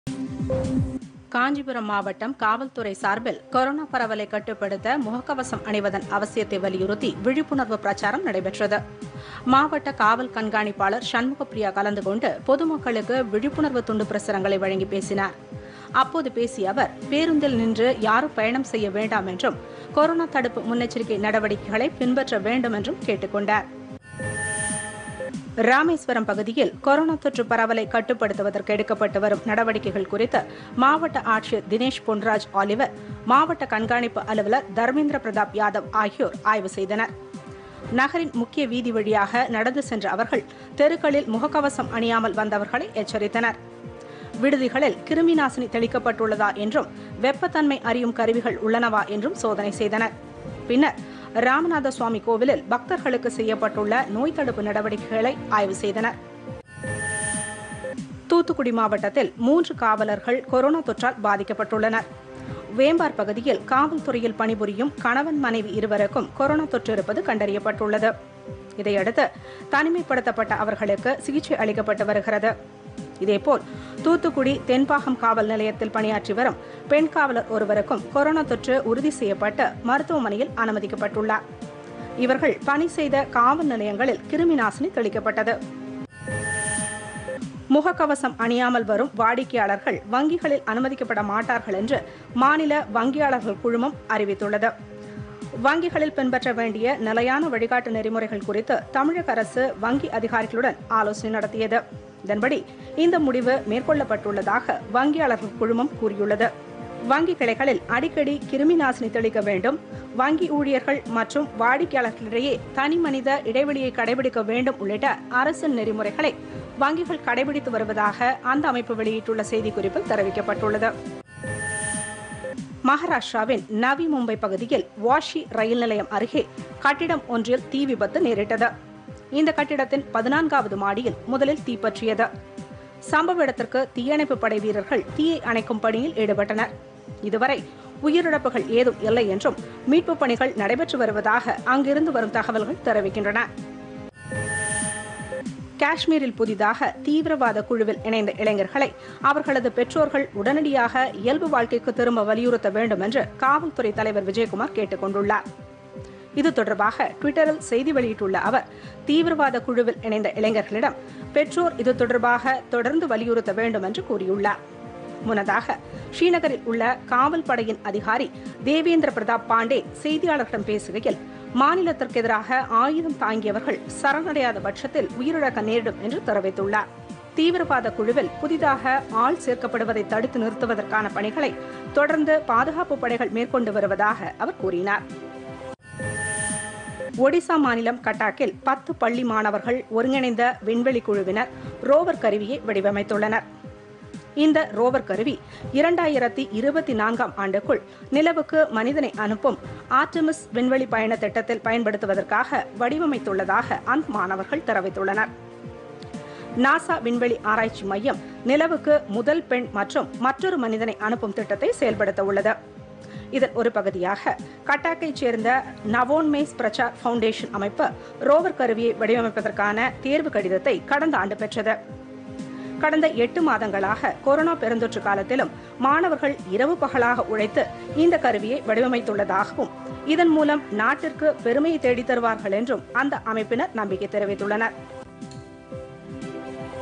பொதிகை காலை 8.00 மணி விரைவுச் செய்திகள் [12.04.2021]
1) கொரோனா விழிப்புணர்வு பிரச்சாரம்

2) அதிகாரிகள் ஆய்வு

3) 3 காவலர்களுக்கு கொரோனா தொற்று

4) வங்கிகளில் முக கவசம் கட்டாயம்

5) தீ விபத்து

6) பெற்றோர்களிடம் காவல்துறை அதிகாரி வேண்டுகோள்

7) பள்ளி மாணவர்கள் வடிவமைத்த ரோவர் கருவி